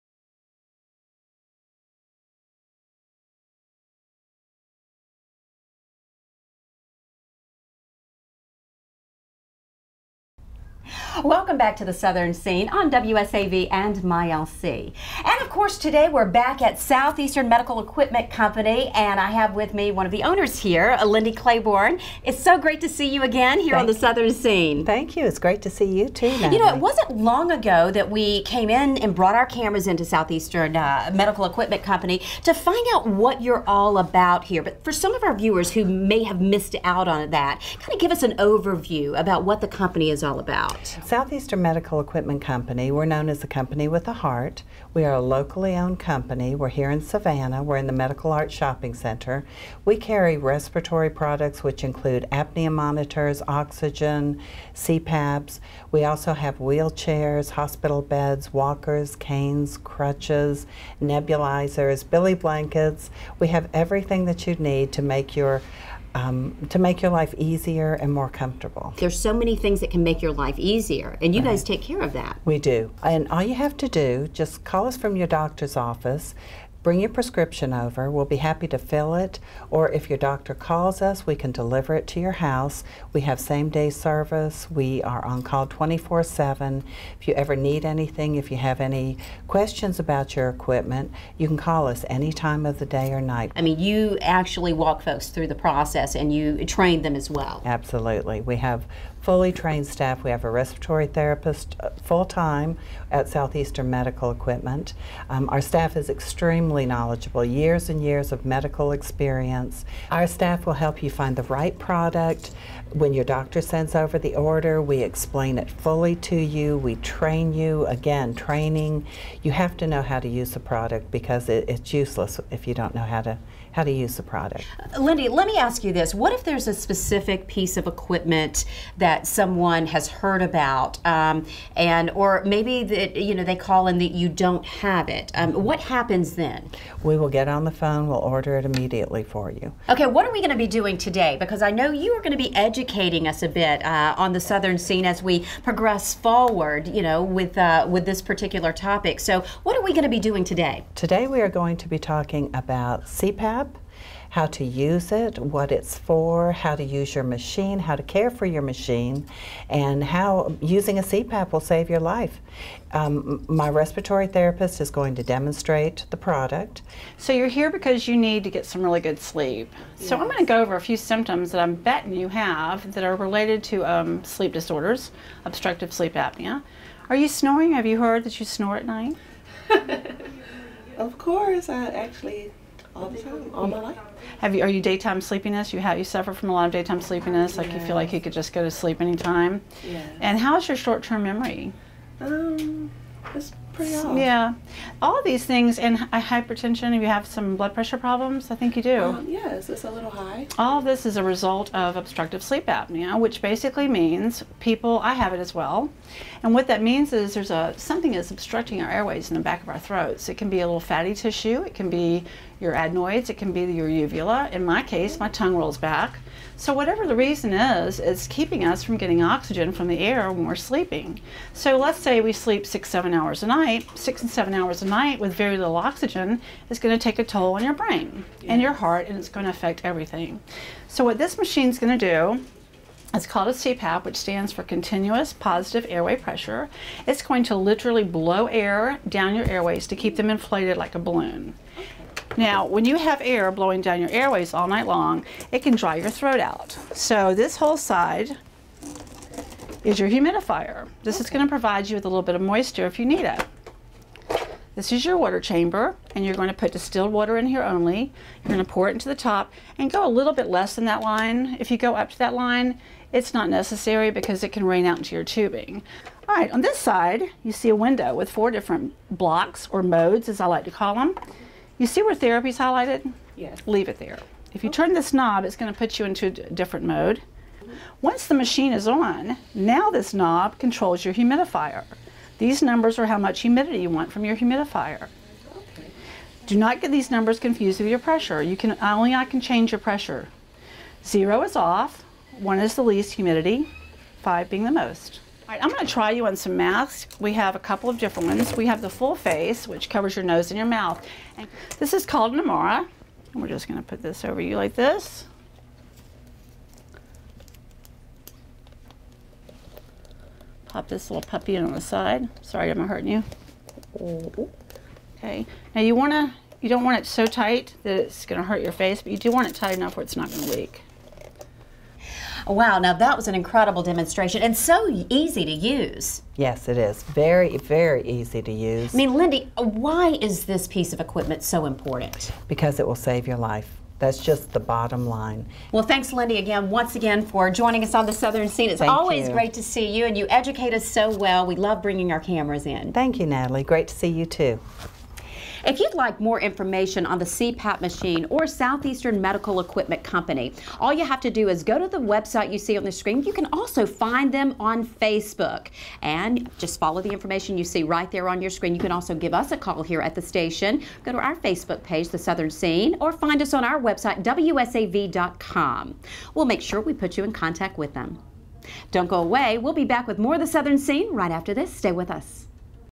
Southeastern Medical Equipment Company (CPAP)
If you have trouble sleeping, you could suffer from sleep apnea. If so, let our good friends at Southeastern Medical Equipment Company demonstrate the latest in CPAP technology!